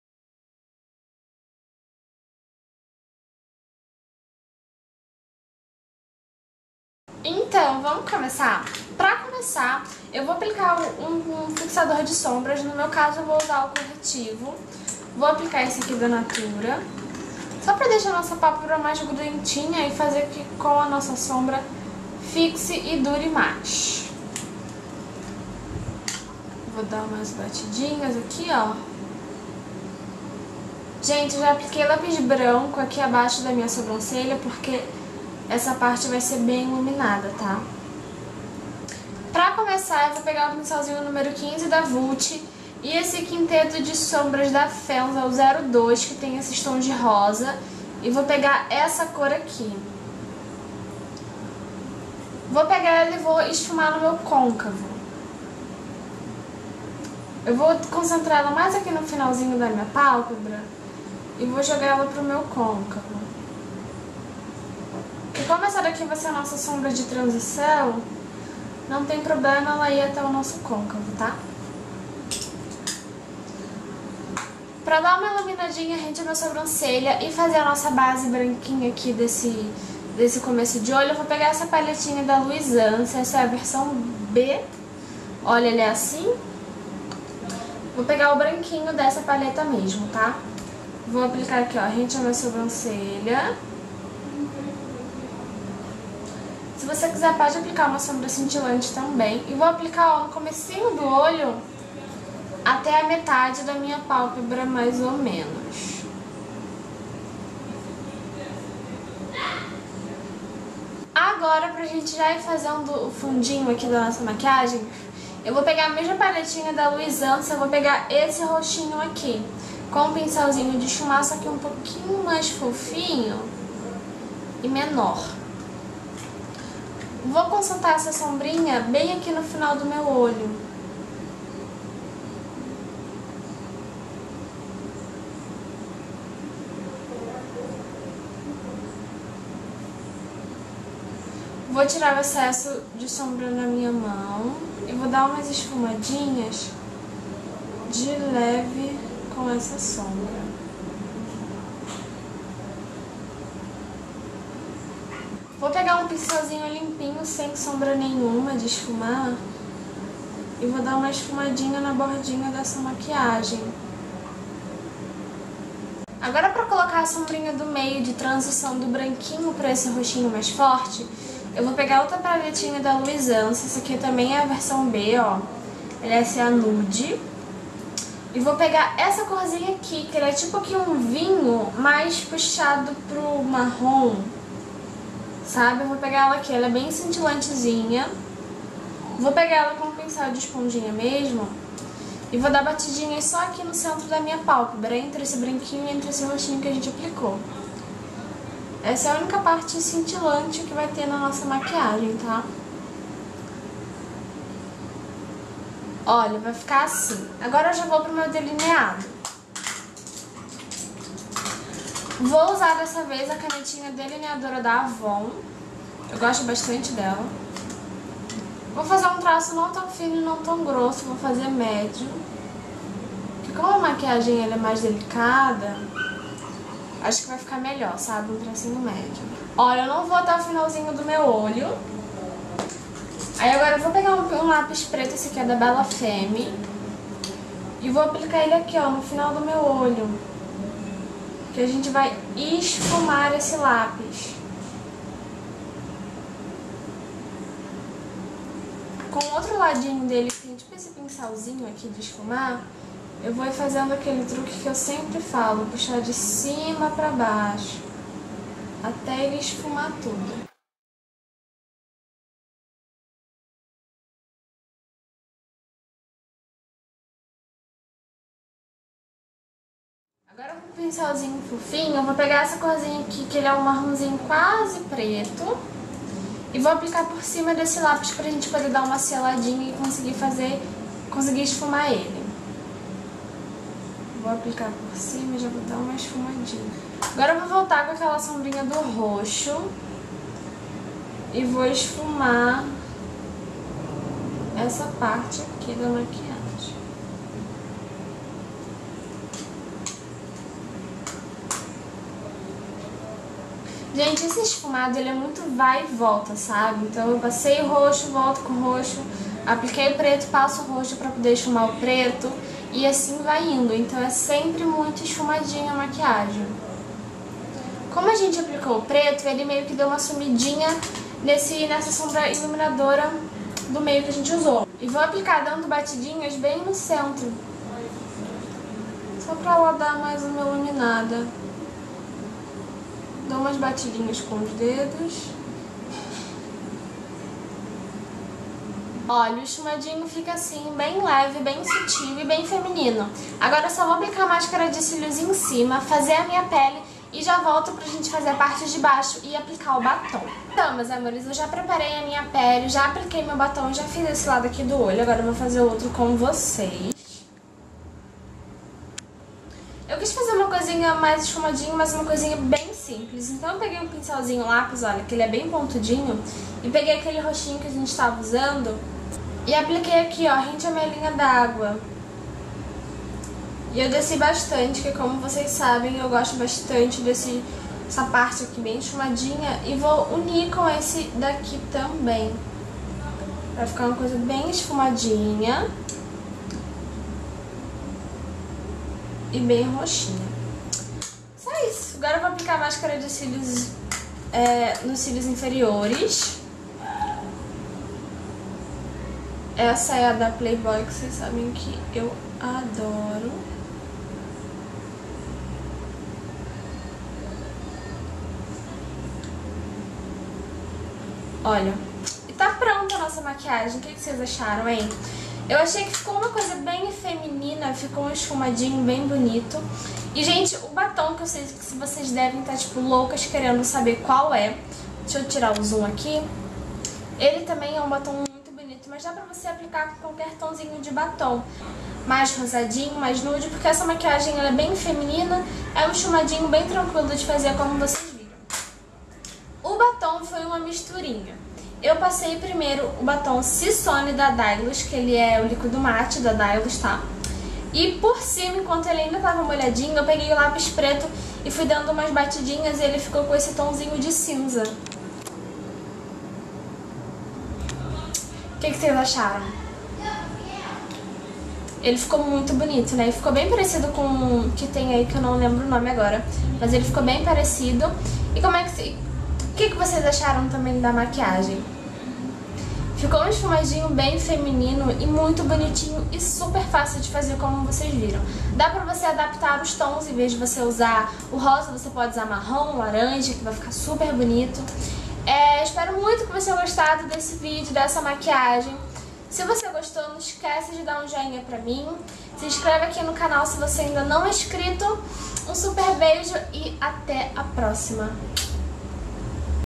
Maquiagem esfumada feminina | Jéssica Juvêncio
Oooooi meus amoresss !!!

Tutorial de maquiagem ja ta no ar , fiz uma maquiagem bem feminina em tons de rosa , e eu espero muito que vocês gostem !!!

BEIJAOOOO